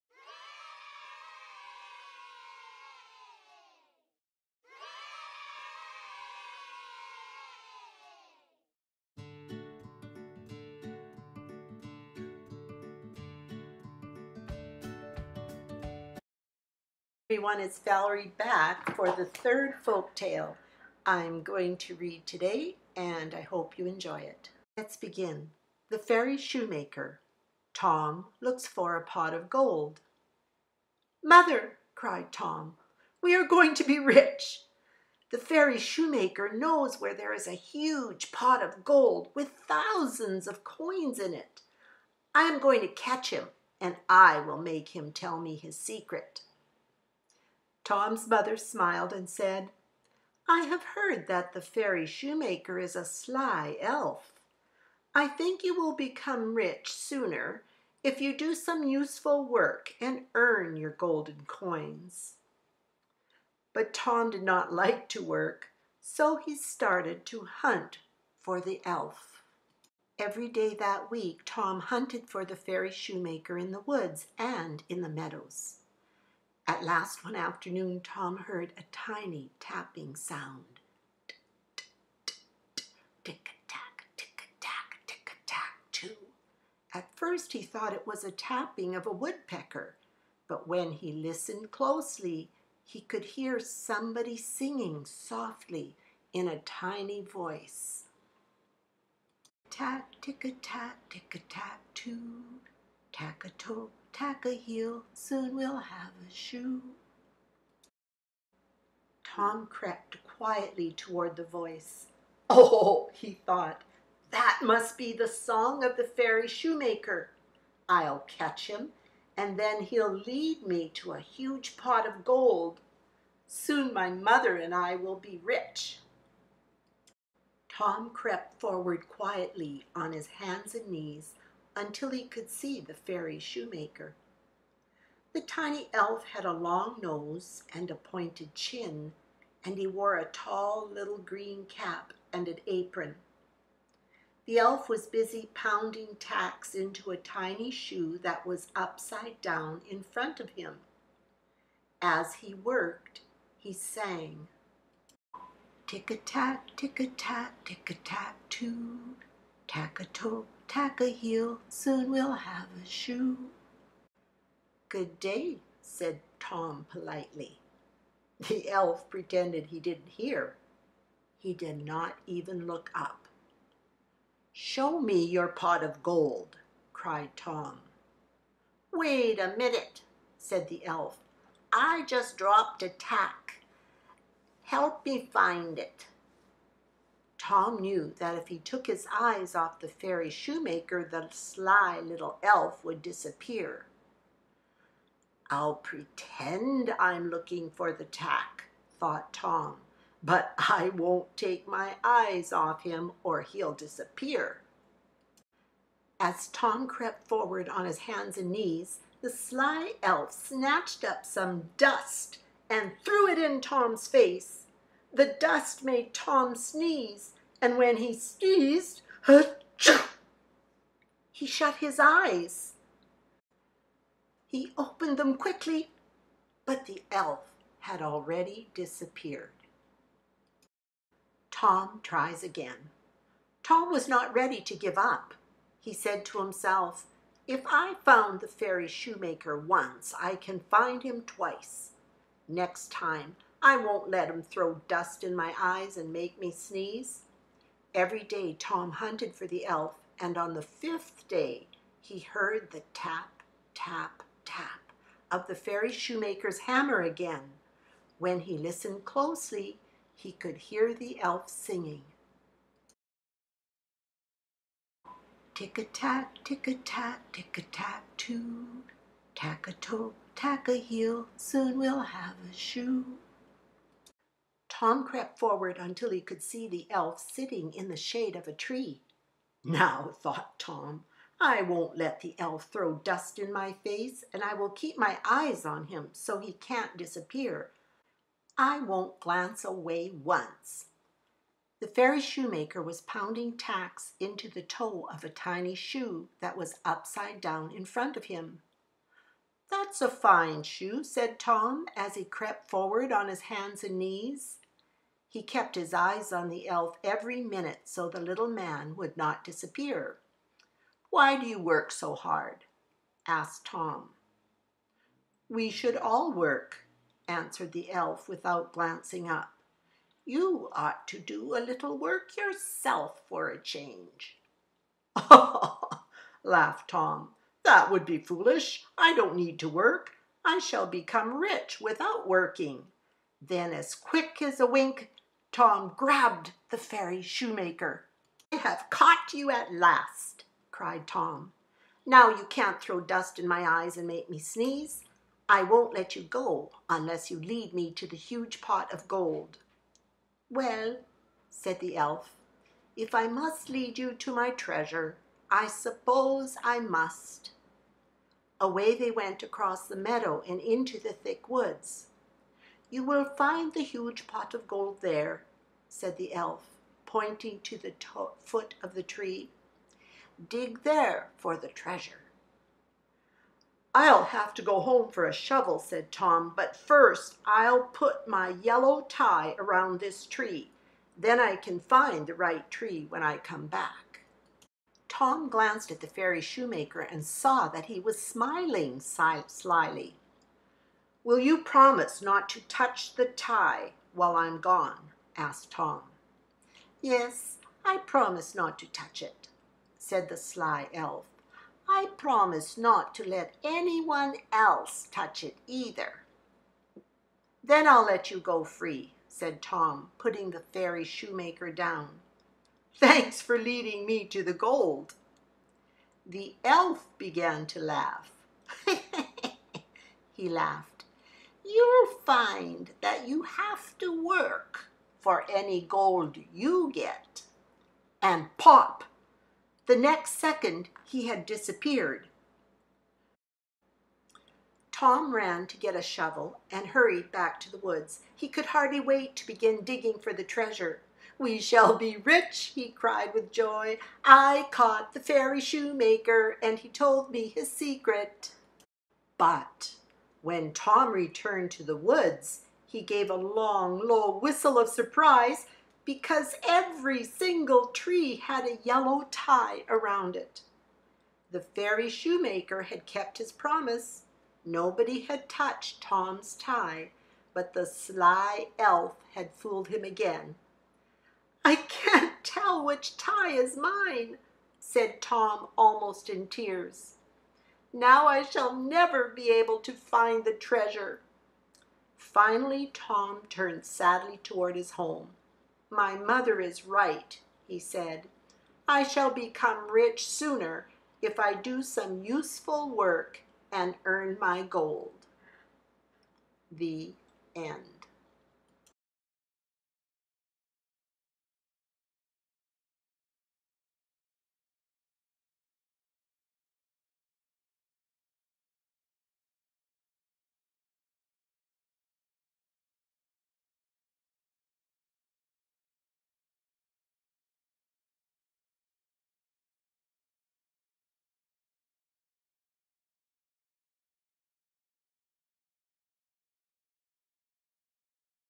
The Fairy Shoemaker- Kids Story #3 Lesson on Work Ethic and Truthfulness
The Fairy Shoemaker is another old Folk Tale that speaks to us about having a good work ethic. This version came from my own third grade reader. Relax, cuddle up, and listen to this Bedtime Story.